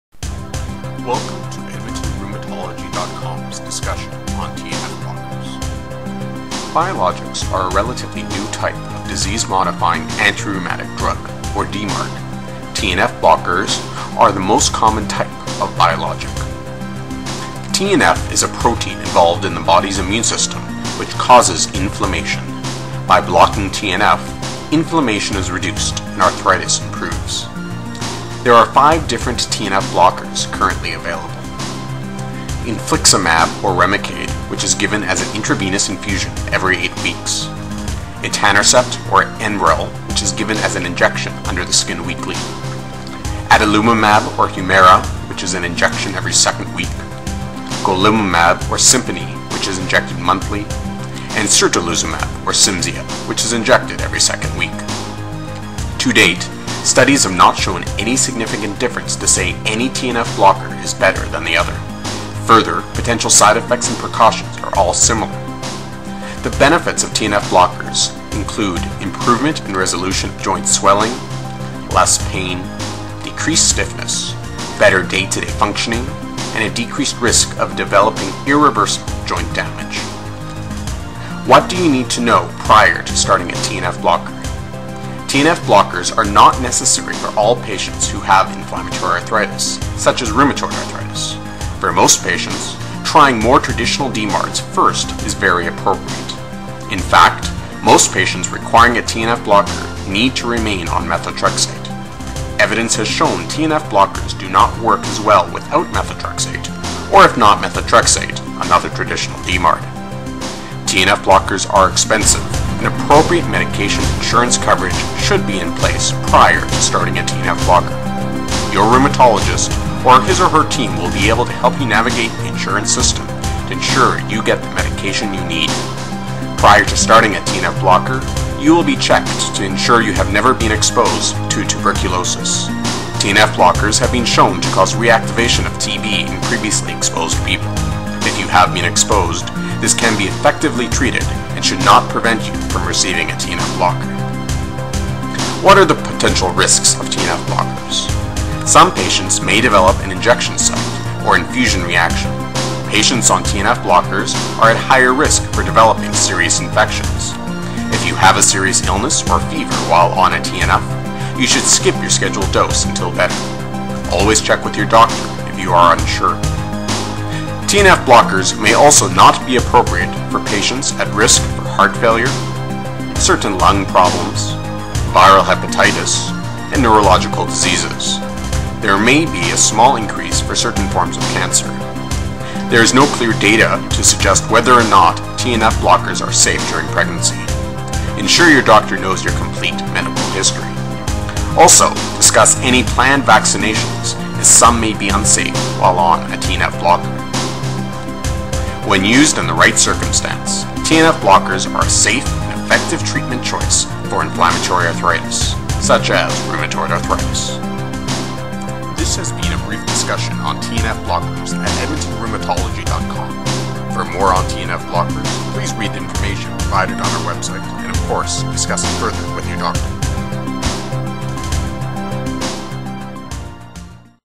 Biologics: TNF Antagonists for Rheumatoid Arthritis
TNF Antagonists are the oldest and the most commonly used biologic for Rheumatoid Arthritis and other types of inflammatory arthritis.  This video briefly reviews their potential benefits and side effects.
Please see our updated video on TNFs.